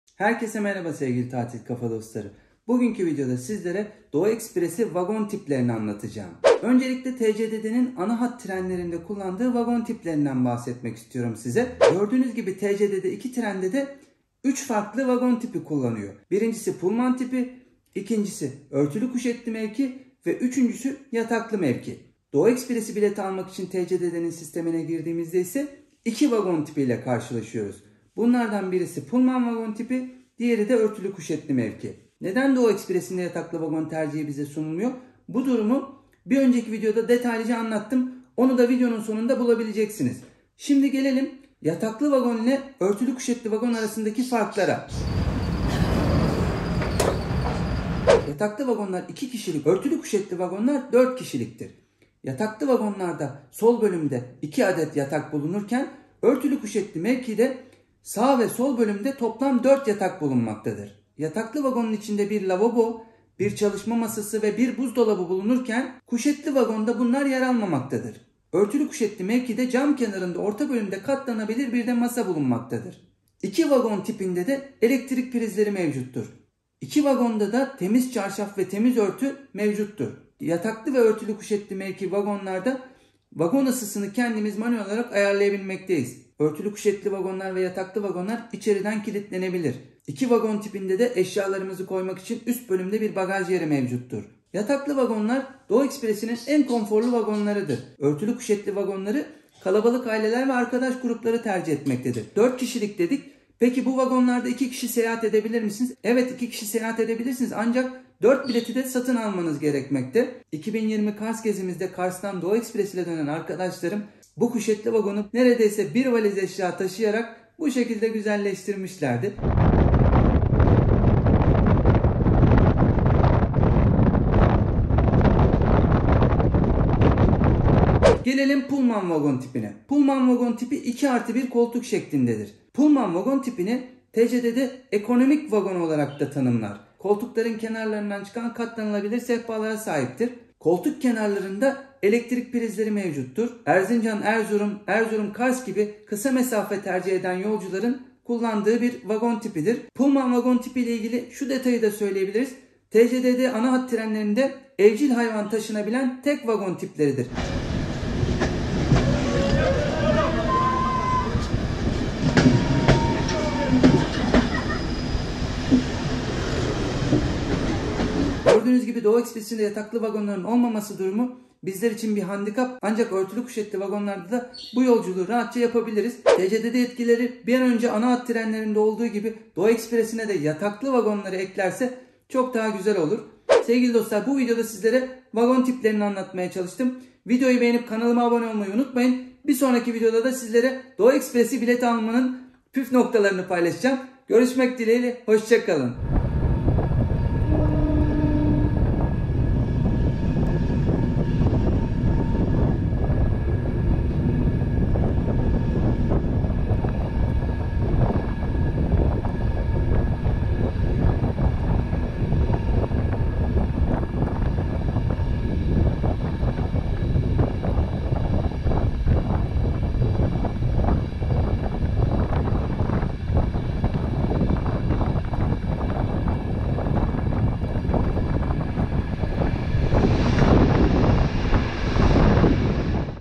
Doğu Ekspresi Vagon Tipleri - Kuşetli Vagon ile Yataklı Vagon Arasındaki Farklar
Merhaba Sevgili Tatil Kafa Dostları

1 Kasım 2021 itibariyle sizlere Doğu Ekspresi'nde bulunan vagon tiplerini detaylıca anlatmaya çalıştım. Tcdd Van Gölü Ekspresi, Güney Ekspresi gibi Anahat trenlerinde kullandığı vagon tiplerinden Yataklı Vagonu Doğu Ekspresi'nde kullanmıyor.
Bunun sebebini de bir önceki videoda detaylıca anlatmıştım.
Bu Linkten İzleyebilirsiniz.

Mevcut durumda bir değişiklik olmazsa bu kış Kars'a Doğu Ekspresi'nin Örtülü Kuşetli ve Pulman Vagon tipleriyle seyahat edebileceğiz.

Örtülü Kuşetli Vagon ve Yataklı Vagon arasındaki farkları da sizlere bu videoda detaylıca anlatmaya çalıştım.

Aklınıza takılan soruları yorum bölümünden sorabilir ya da Tatil Kafa instagram hesabından bana yazabilirsiniz.

İyi Seyirler.